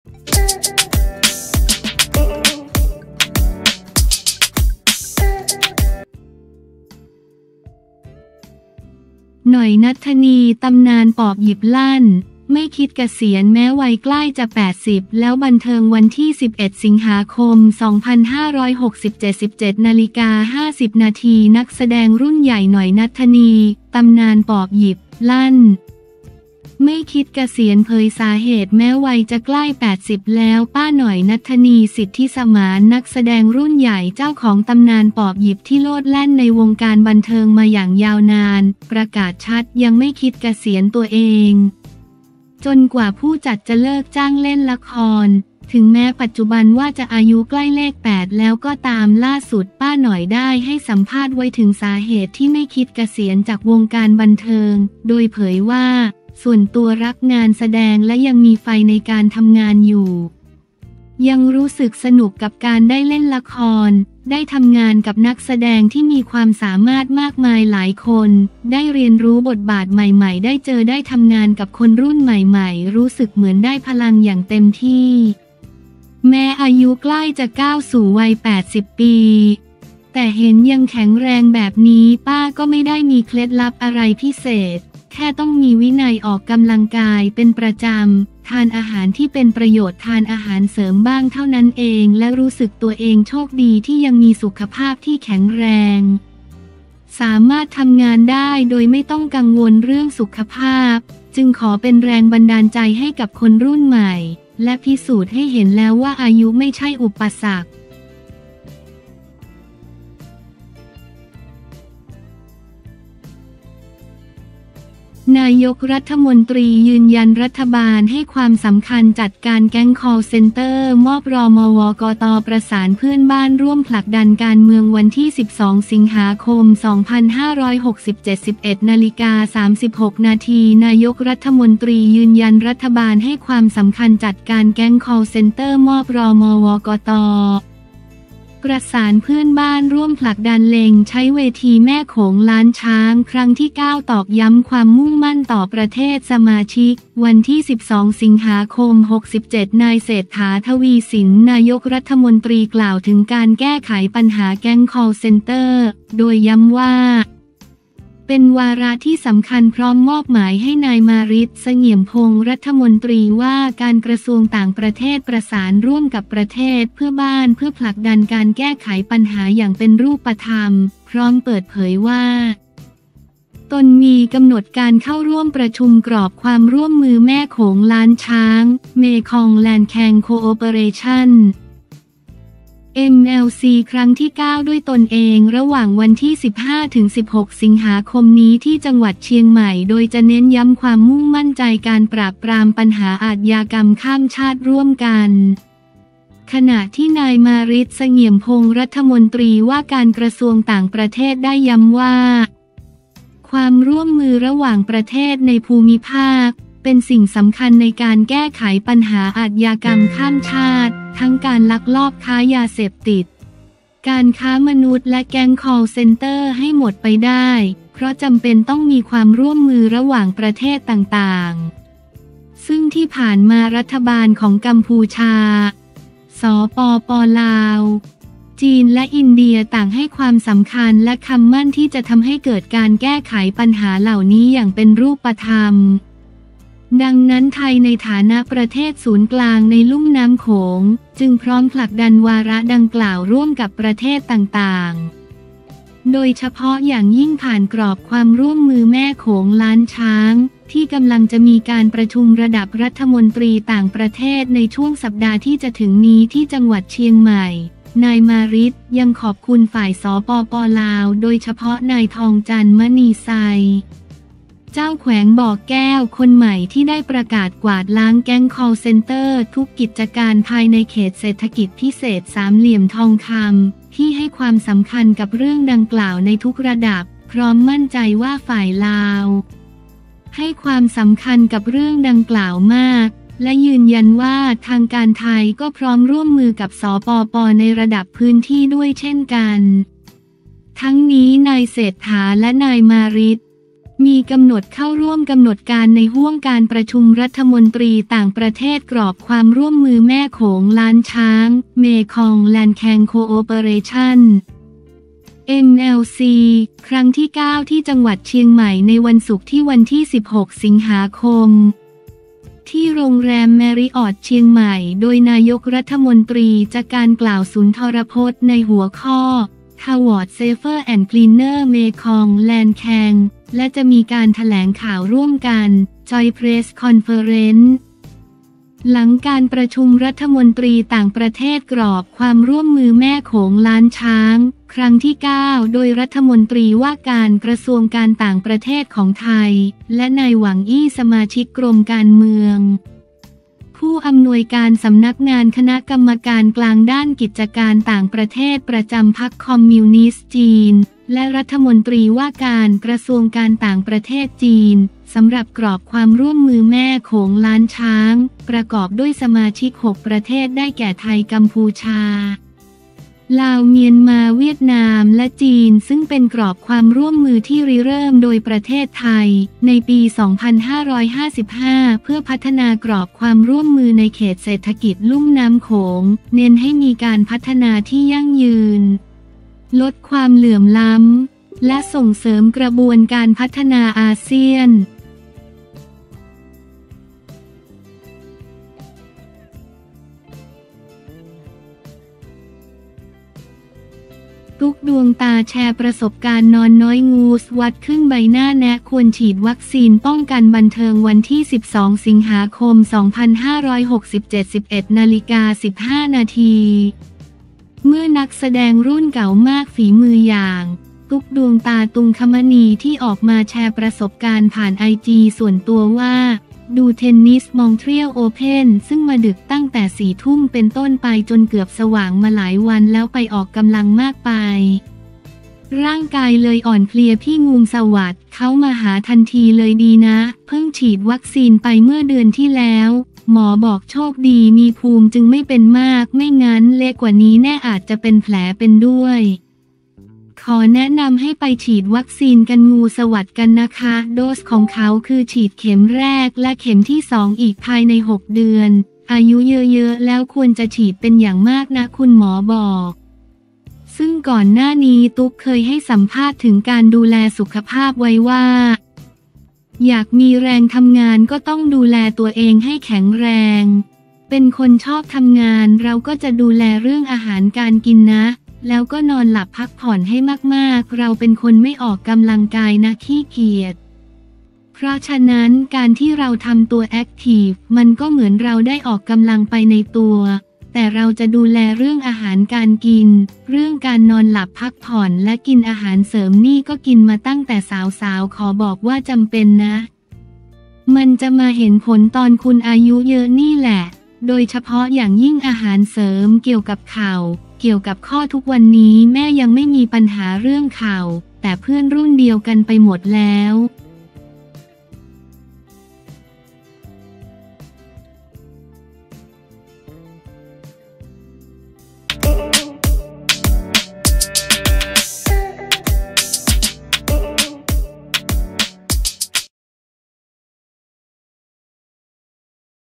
หน่อย ณัฐนี ตำนานปอบหยิบ ลั่น! ไม่คิดเกษียณ แม้วัยใกล้จะ 80 แล้ว
11 ส.ค. 2567
17:50 น.
นักแสดงรุ่นใหญ่ หน่อย ณัฐนี “ตำนานปอบหยิบ” ลั่น! ไม่คิดเกษียณ เผยสาเหตุแม้วัยจะใกล้ 80 แล้ว

ป้าหน่อย ณัฐนี สิทธิสมาน นักแสดงรุ่นใหญ่ เจ้าของตำนานปอบหยิบ ที่โลดแล่นในวงการบันเทิงมาอย่างยาวนาน ประกาศชัดยังไม่คิดเกษียณตัวเอง จนกว่าผู้จัดจะเลิกจ้างเล่นละคร ถึงแม้ปัจจุบันว่าจะอายุใกล้เลข 8 แล้วก็ตาม

ล่าสุด ป้าหน่อย ได้ให้สัมภาษณ์ไว้ถึงสาเหตุที่ไม่คิดเกษียณจากวงการบันเทิง โดยเผยว่า “ส่วนตัวรักงานแสดง และยังมีไฟในการทำงานอยู่ ยังรู้สึกสนุกกับการได้เล่นละคร ได้ทำงานกับนักแสดงที่มีความสามารถมากมายหลายคน ได้เรียนรู้บทบาทใหม่ๆ ได้เจอได้ทำงานกับคนรุ่นใหม่ๆ รู้สึกเหมือนได้พลังอย่างเต็มที่

“แม้อายุใกล้จะก้าวสู่วัย 80 ปี แต่เห็นยังแข็งแรงแบบนี้ ป้าก็ไม่ได้มีเคล็ดลับอะไรพิเศษ แค่ต้องมีวินัยออกกำลังกายเป็นประจำ ทานอาหารที่เป็นประโยชน์ ทานอาหารเสริมบ้างเท่านั้นเอง และรู้สึกตัวเองโชคดีที่ยังมีสุขภาพที่แข็งแรง สามารถทำงานได้โดยไม่ต้องกังวลเรื่องสุขภาพ จึงขอเป็นแรงบันดาลใจให้กับคนรุ่นใหม่ และพิสูจน์ให้เห็นแล้วว่า อายุไม่ใช่อุปสรรค